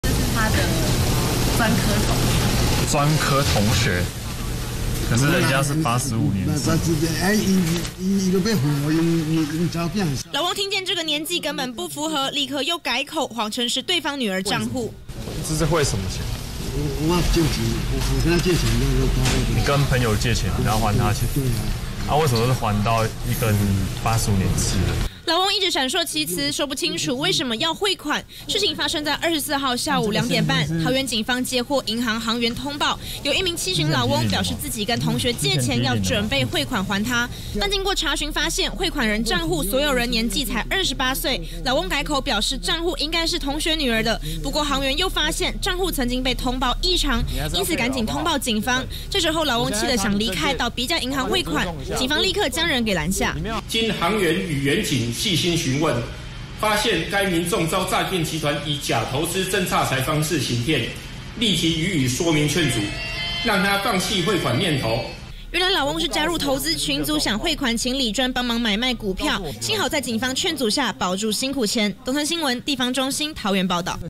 七旬老翁遭投資詐騙 堅持要匯款給28歲「同學」@newsebc
桃園一名老翁，遭到投資詐騙，跑到銀行要匯錢，行員一問要匯給誰，老翁說同學，但一問同學才28歲，怎麼可能，趕快報警阻詐。

11/10~11/24 敬請鎖定51頻道！
⚾場次看這邊⚾
11/13 18:25 中華VS韓國
11/14 18:25 中華VS多明尼加